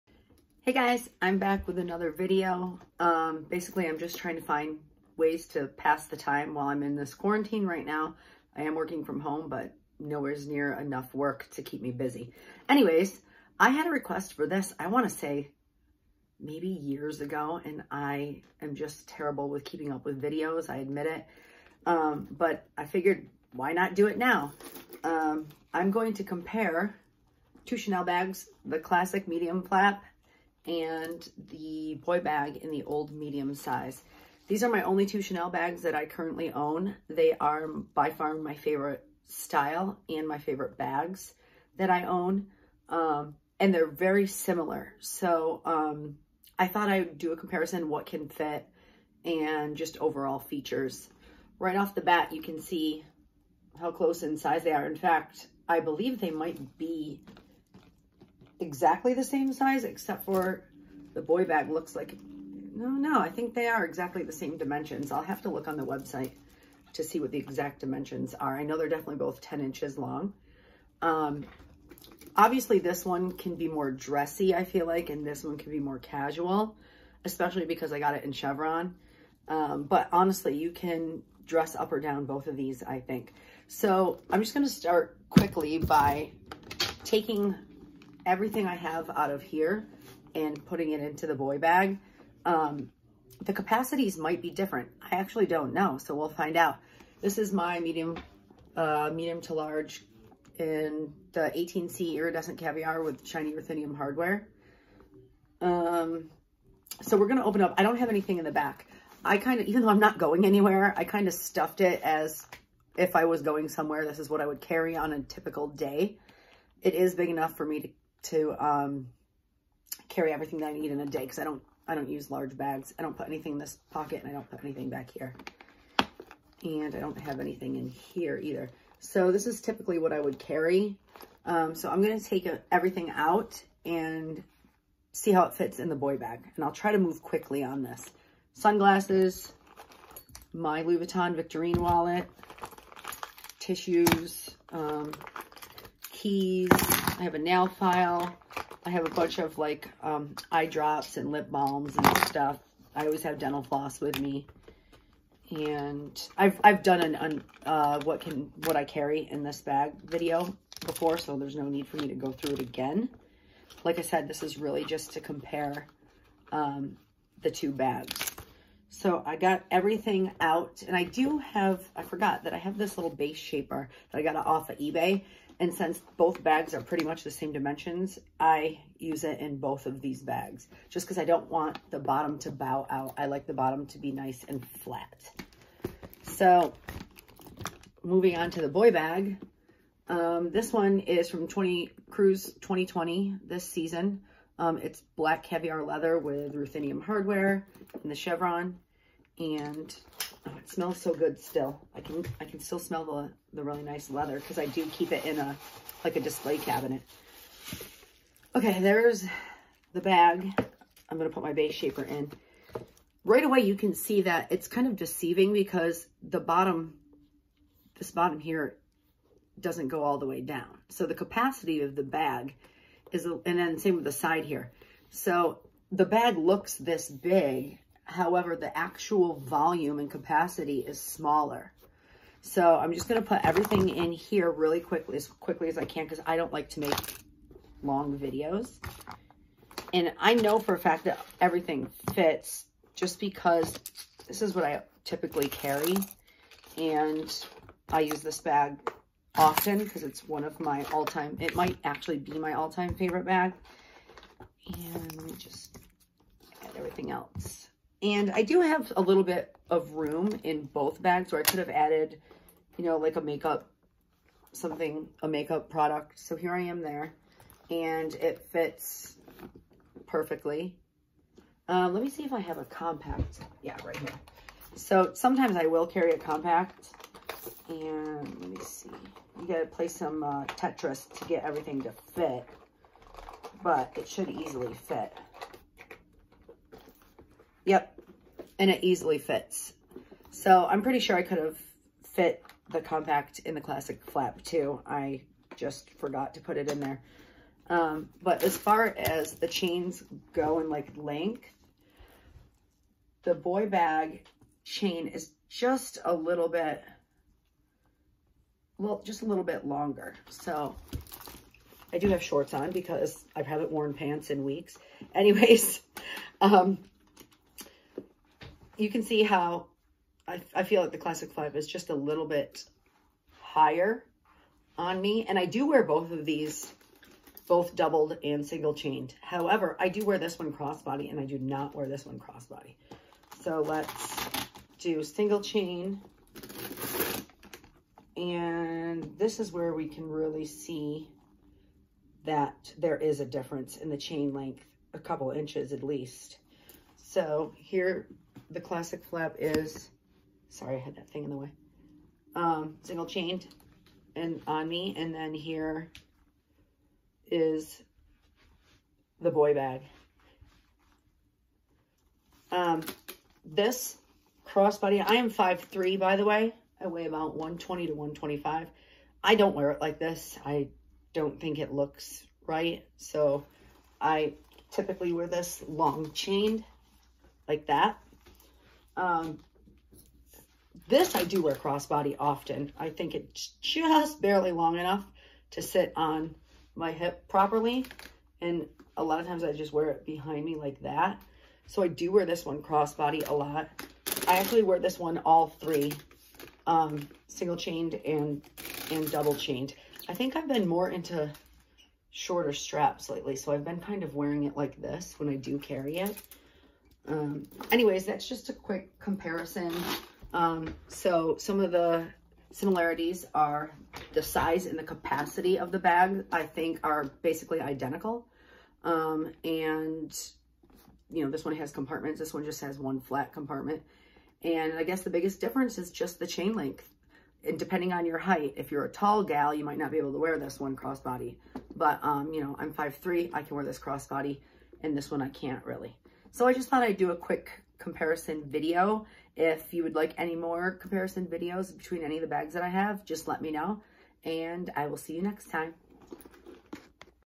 Comparison between Chanel Classic Flap and Boy bag
Hey guys! In this video I’m comparing two Chanel bags that are very similar in size and shape.